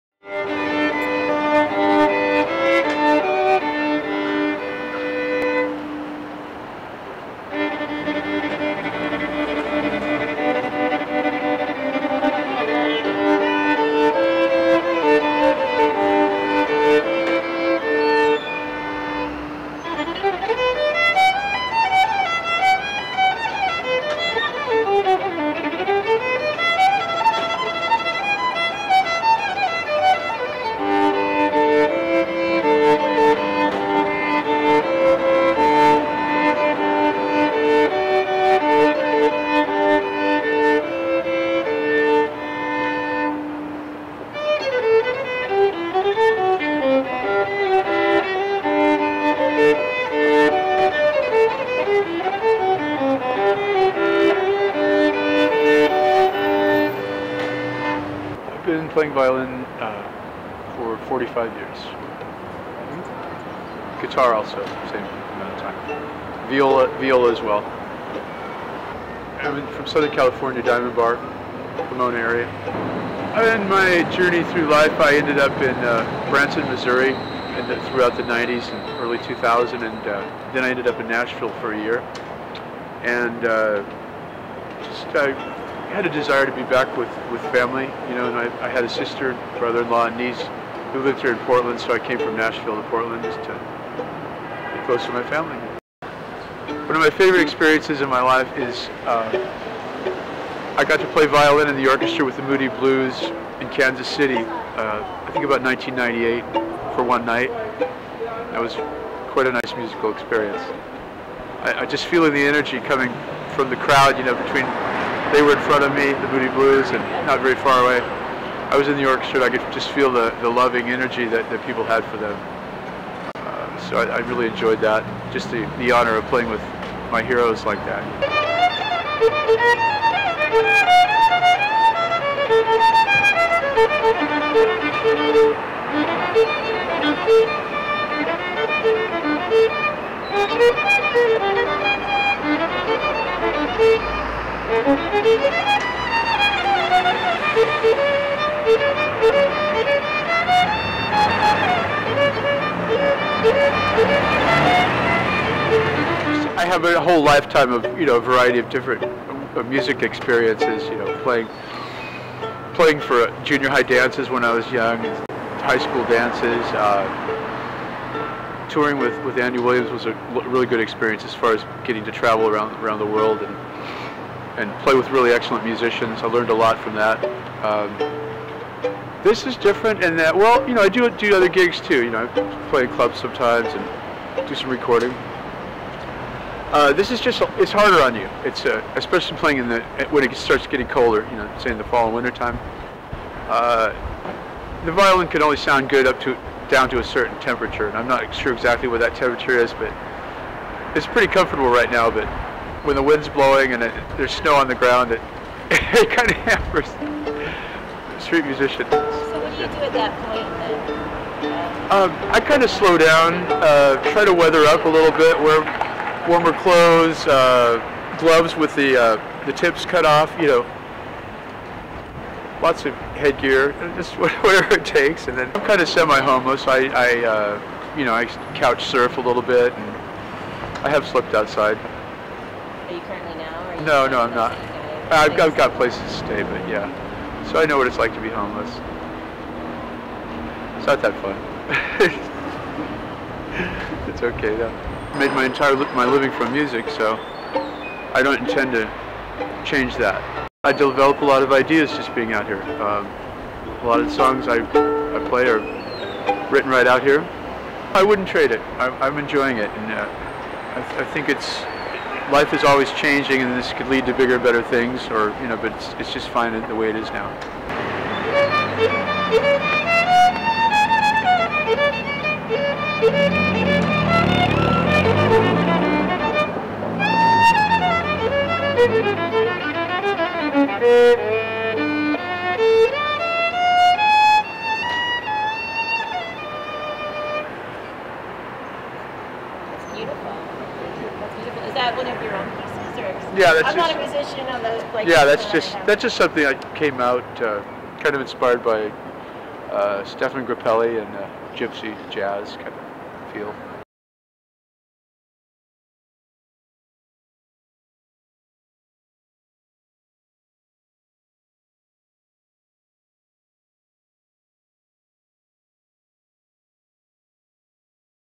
ABOTC online| 2009 |Windham Bruce Violinist
Artbeat of the City: An ongoing ethnographic documentary series about street artists and performers in Portland, Oregon
Bruce Windham
Violinist, guitarist and viola player
Bruce is originally from Diamond Bar, California. Bruce moved to Orgeon, after years of traveling and living in other states, to be near his family members who lived in Portland. On this day, in October of 2009. Bruce was performing in front of Powell's Bookstore. By the year 2009, he had been playing violin, guitar and viola for 45 years. "I've never had another job" said Bruce, "I made my entire living from music, so I don't intend on changing that." Bruce has played all over the United States and the world. His favorite memory was when he got to perform in the orchestra for a Moody Blues concert around 1999.

This is a single interview and moment on the street with Bruce. Prior to filming, I had heard him playing throughout Portland several times and his sound always drew me in. We spoke several times, prior to the interview, about his work and the ongoing project, Artbeat of the City. Thank you Bruce for sharing your beautiful gift with Portland.

Interviewer, cameraperson, editor: Mary Anne Funk
©Mary Anne Funk/Artbeat of the City